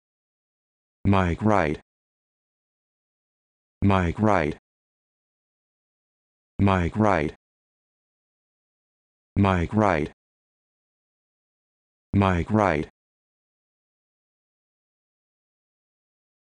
How to Pronounce Micrite
This video shows you how to pronounce Micrite